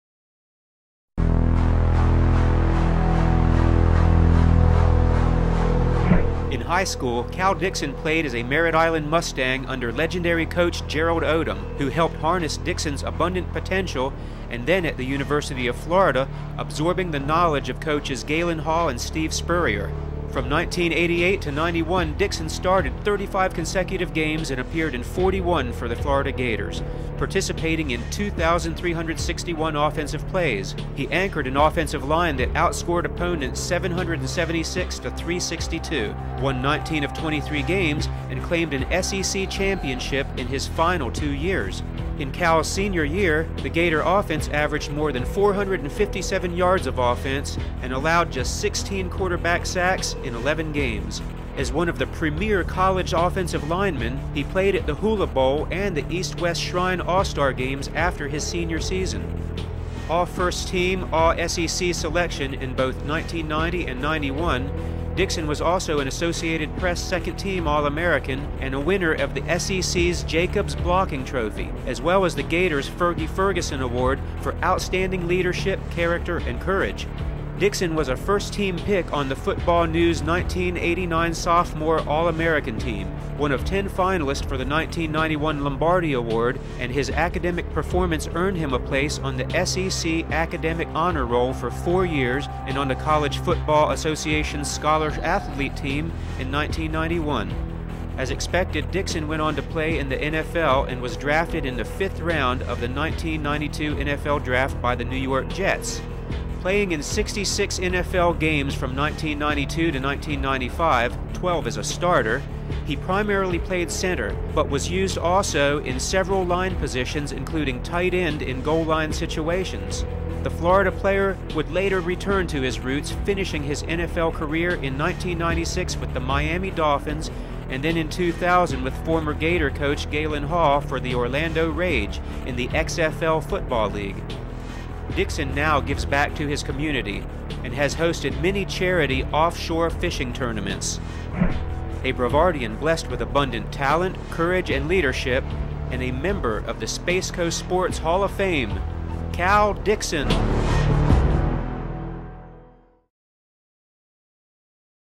Cal Dixon: Space Coast Sports Hall of Fame 2014
Ironman Cal Dixon Anchors O-Line For Gators

At the University of Florida, Cal Dixon started 35 consecutive games for the Gators and appeared in 41 during the 1988 to 1991 seasons, participating in 2,361 offensive plays during his career. He anchored an offensive line that outscored opponents 776 to 362, won 19 of 23 games and claimed an SEC championship in his final two years.